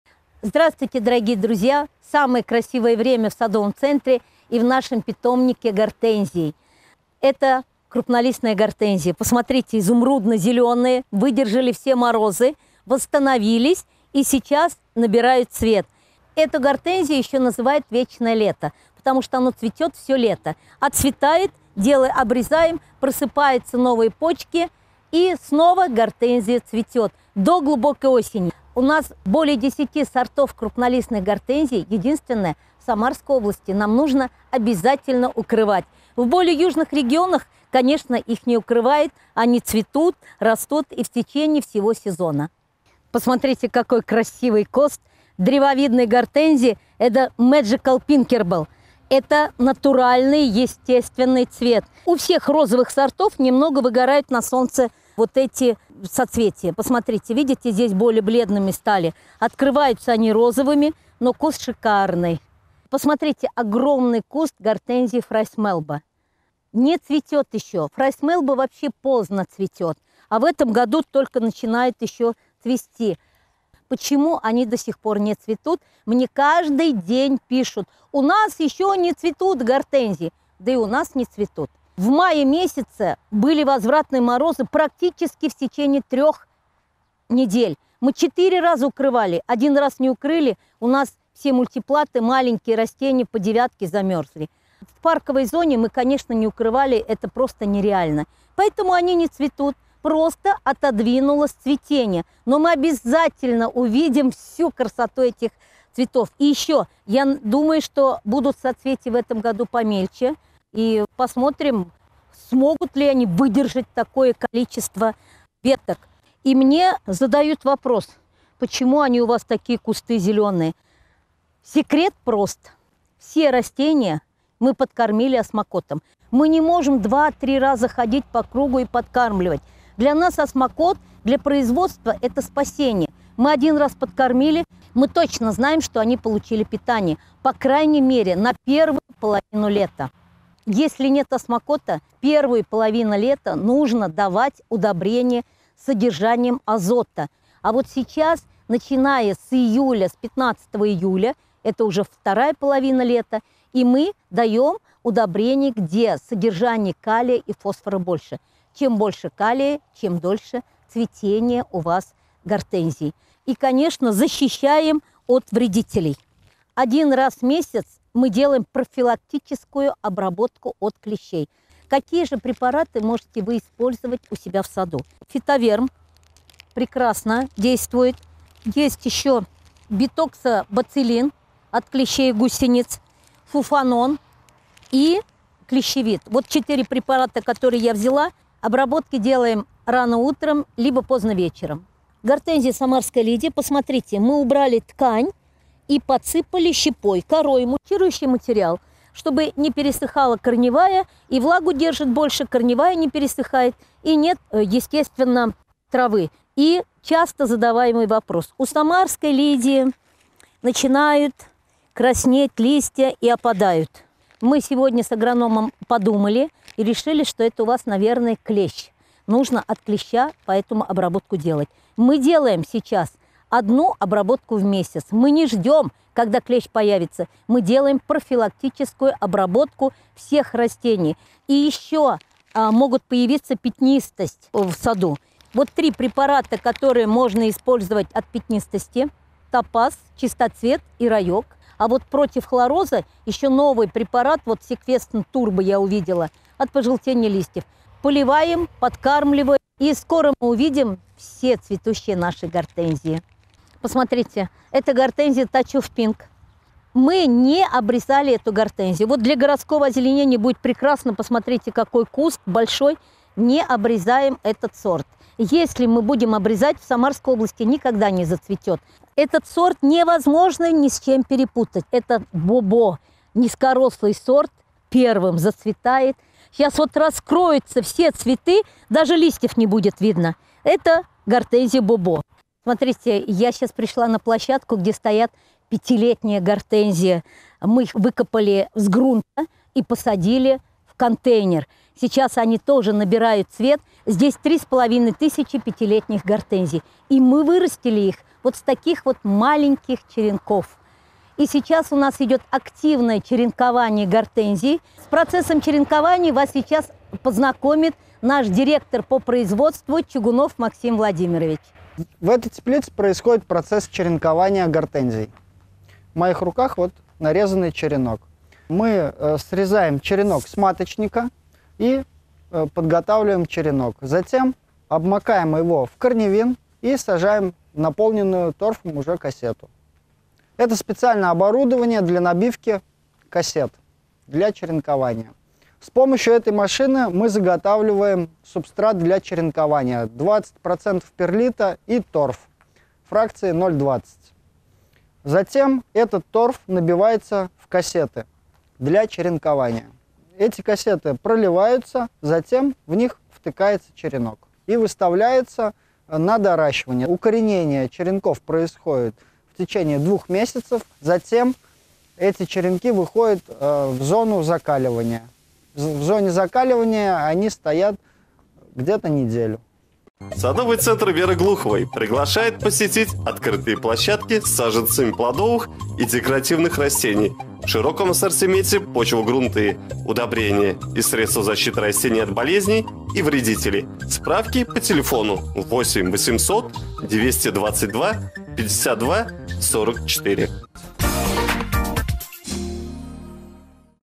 Зеленое черенкование гортензий, подкормки и обработки от болезней и вредителей в питомнике Глуховой.
Предлагаем Вашему вниманию видеосюжет о зеленом черенковании гортензий, подкормках и обработках от болезней и вредителей в питомнике Веры Глуховой.